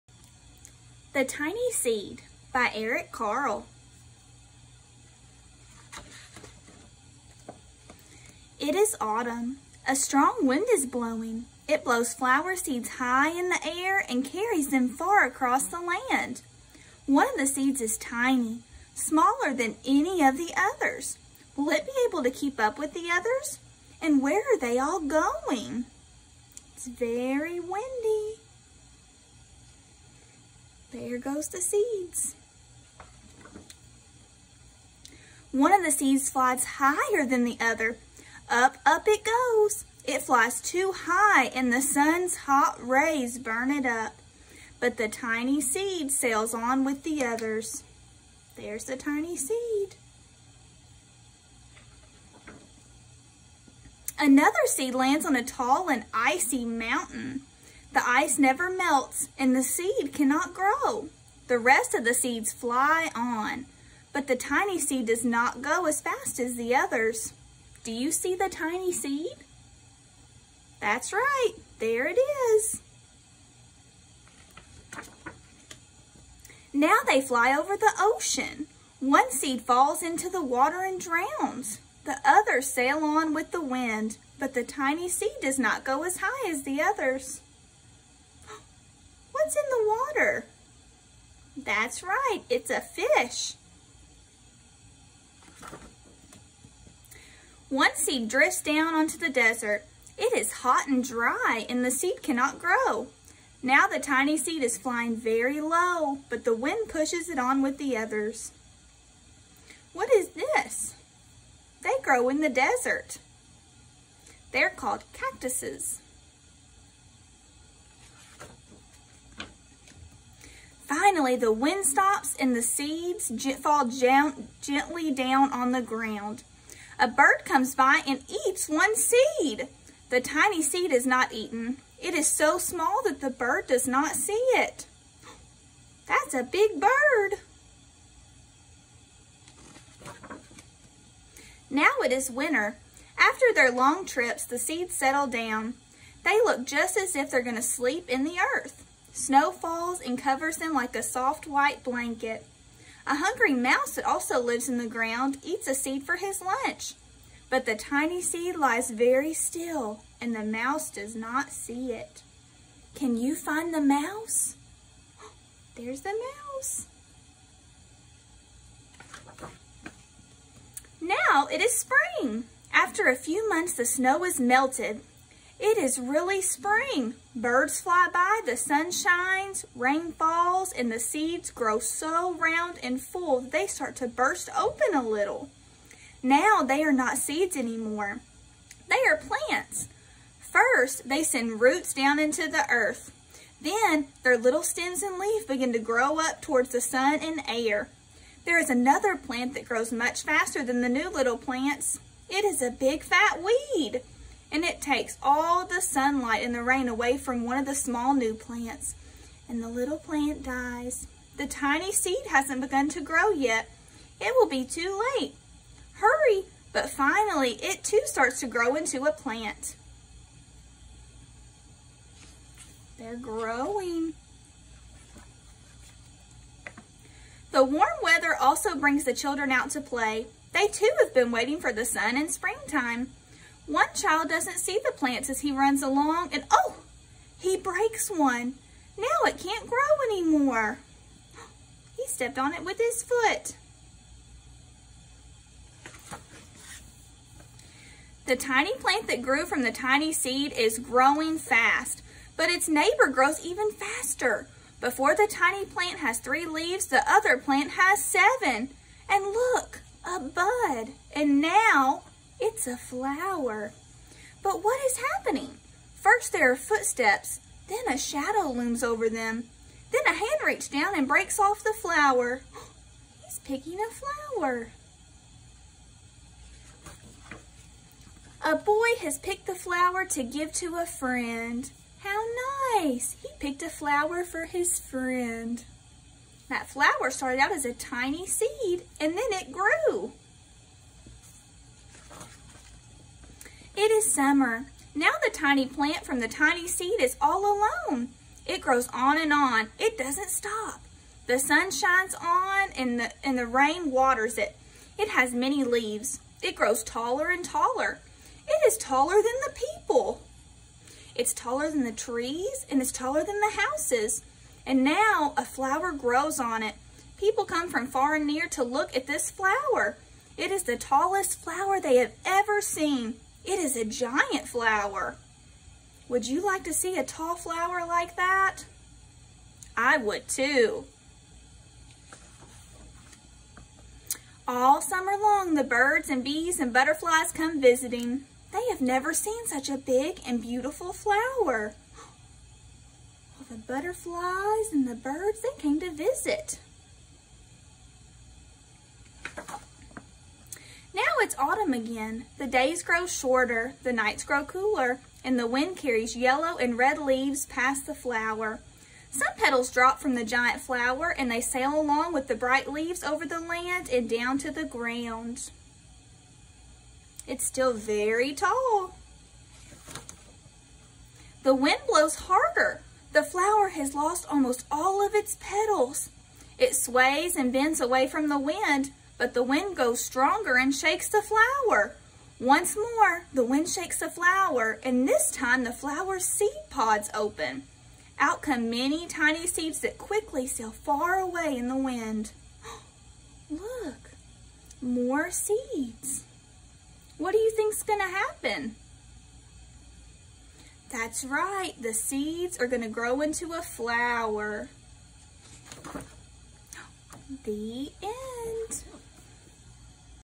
The Tiny Seed
Follow along to the story to see how a tiny seed turns into a tall flower!
Click the provided link to access the activities we have planned for this weeks topic, "Let's Plant a Garden:"